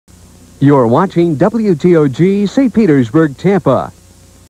WTOG Station ID 1986
This is WTOG's station ID from 1986
WTOG is located in St. Petersburg, FL